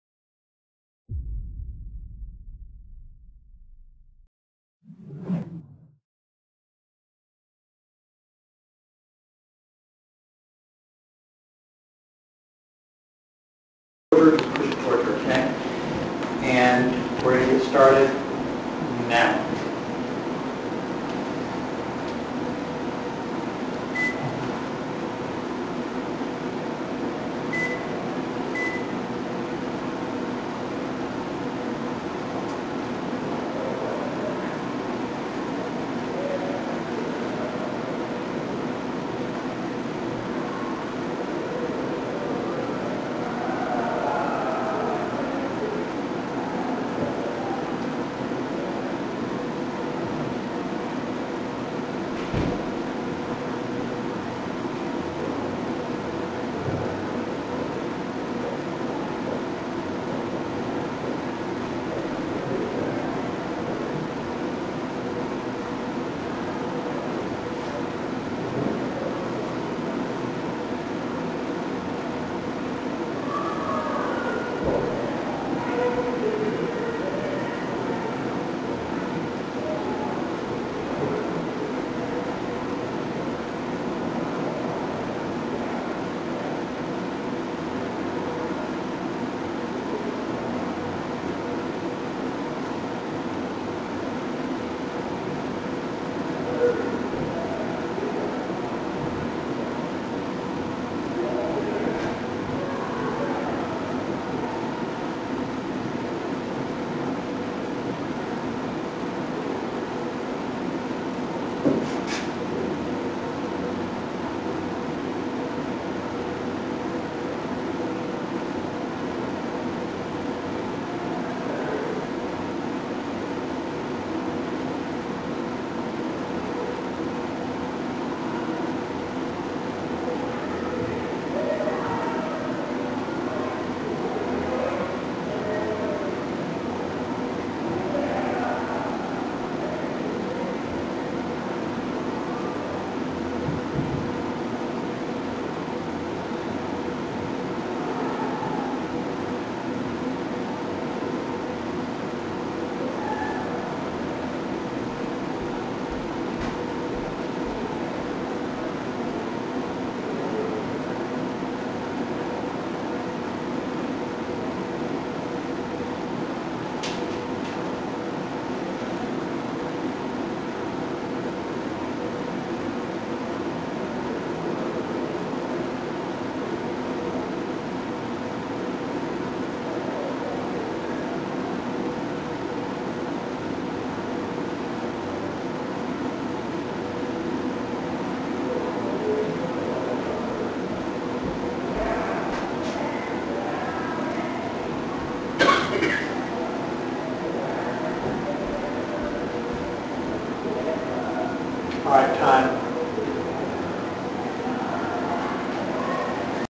EVP From Basement of Moon River Brewery
This was recorded Halloween night 2009 in the basement of Moon River Brewery. From the three beeps in the beginning (someone cutting on the recorder) to the cough at the end...no one says a word.